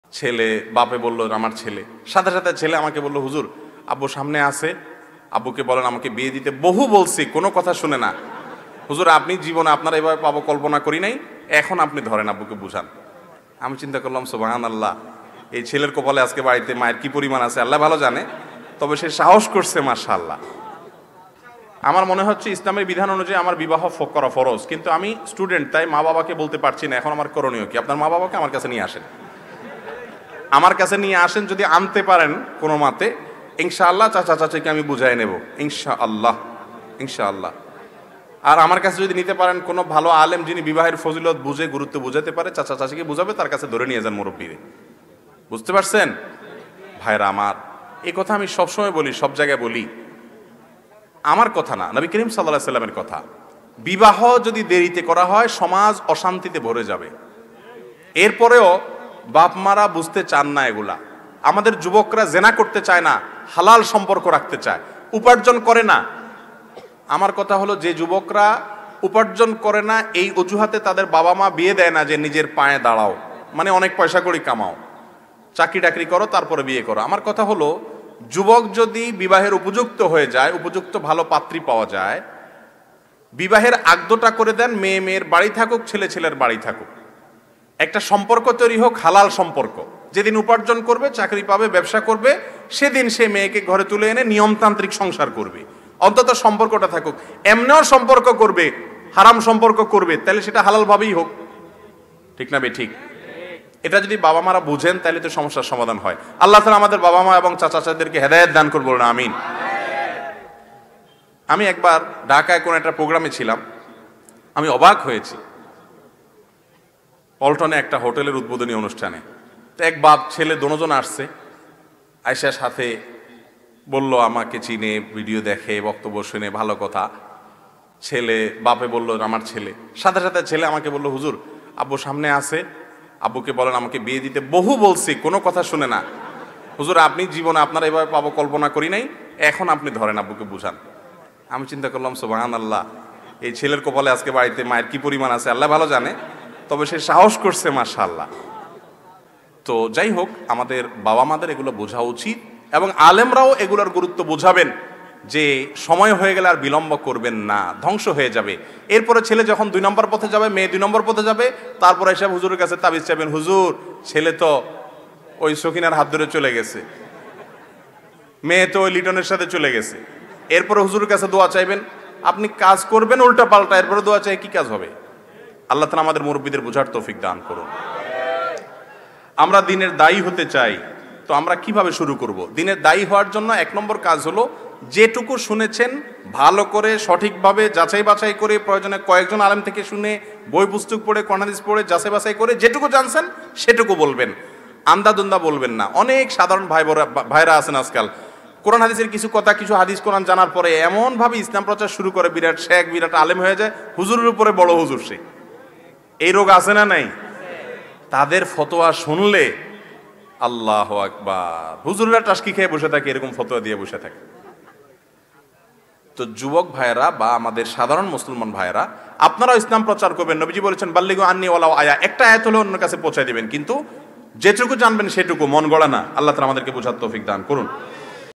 স্টুডেন্ট হয়ে বিয়ে করতে চায় বাবা করায় না তাই হুজুরকে পেয়ে বাবাকে বিয়েতে রাজি করাতে বললো😊
স্টুডেন্ট হয়ে বিয়ে করতে চায় বাবা করায় না তাই হুজুরকে পেয়ে বাবাকে বিয়েতে রাজি করাতে বললো😊 বাকিটা শুনুন। শায়খ আহমাদুল্লাহ। Sheikh Ahmadullah